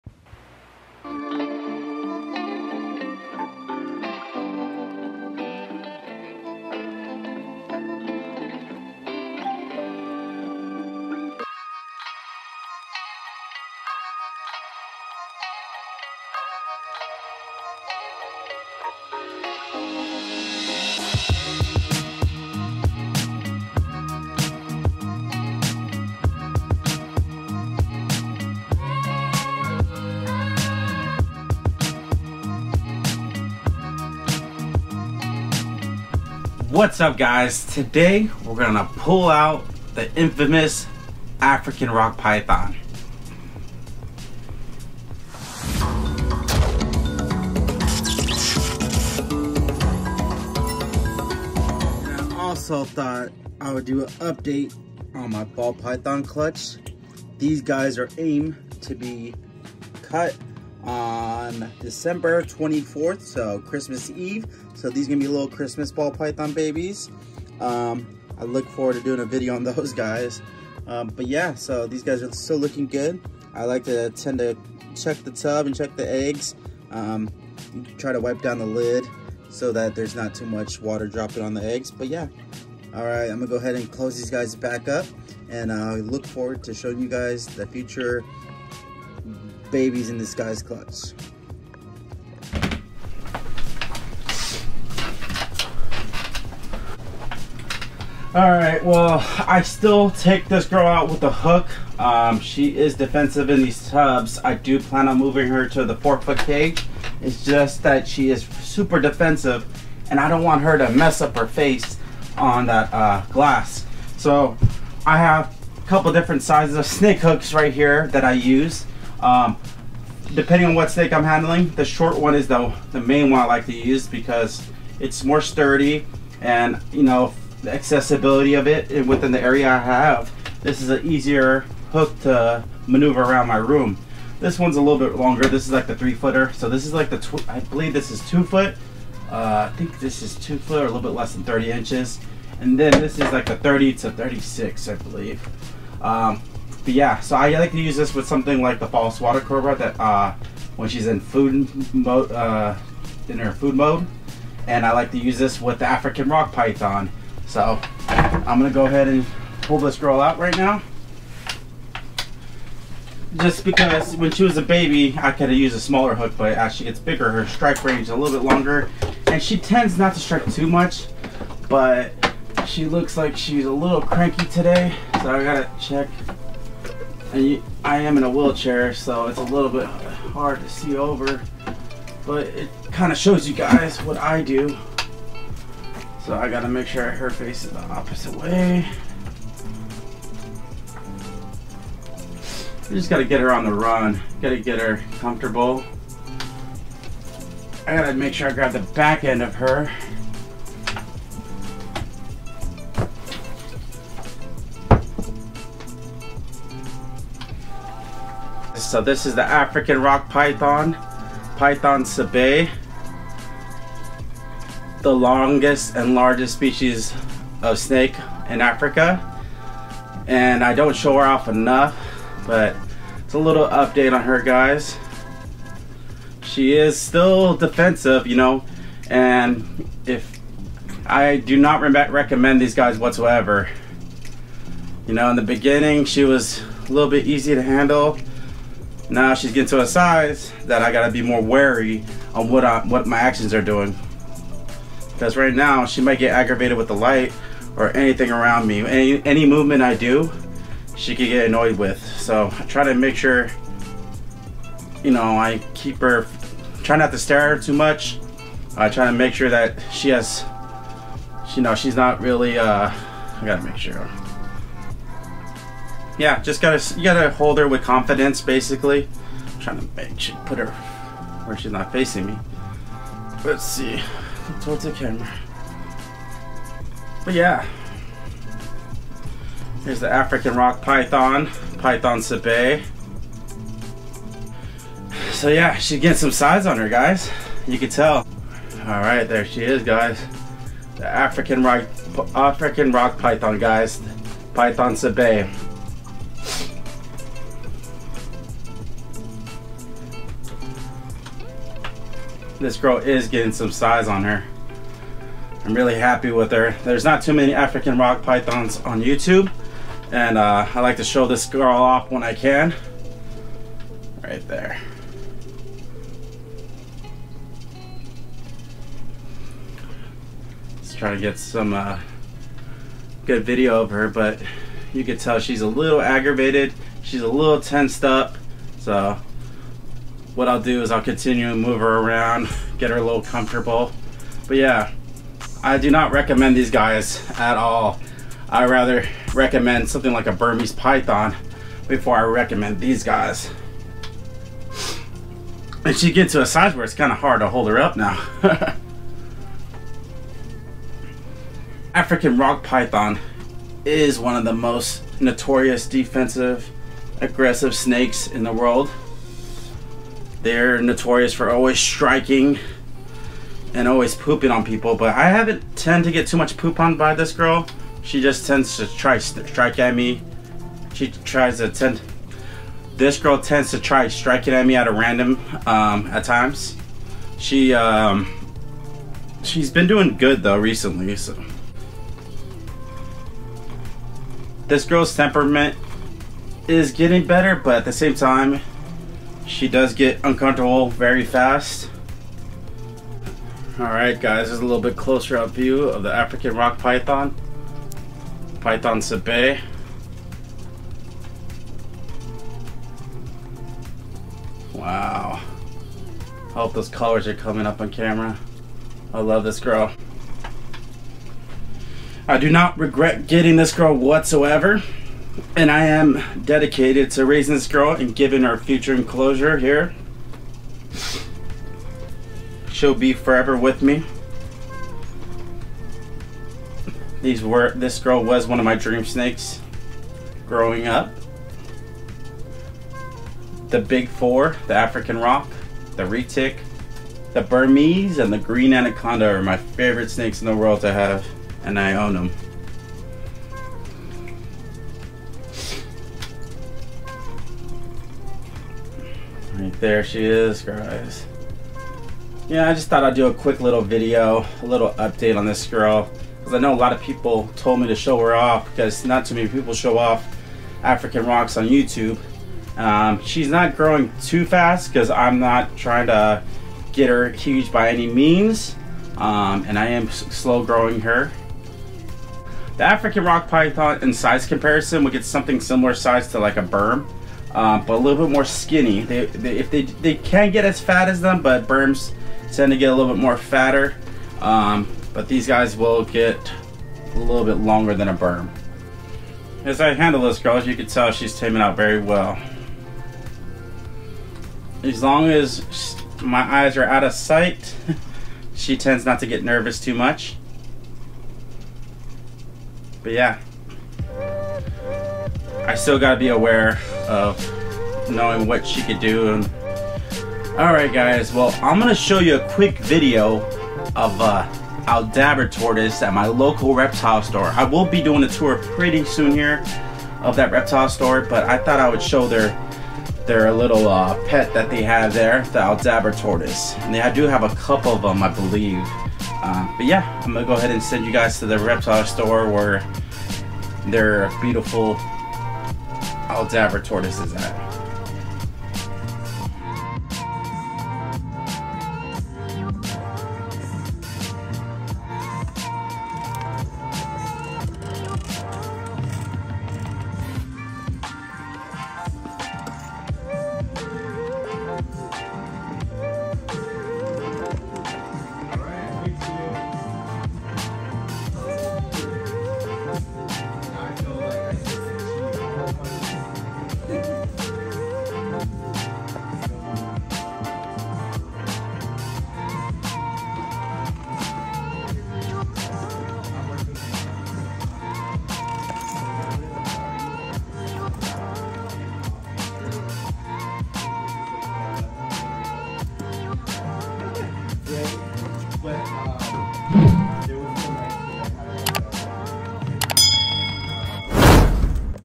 Handling Africa’s Largest Snake the African Rock Python | Free Roaming Giant Aldabra Tortoise
I update you guys on Africa’s Largest snake the African Rock Python and show my local reptile shops pet the giant Aldabra Tortoise!

Channel Instagram : bionic_benny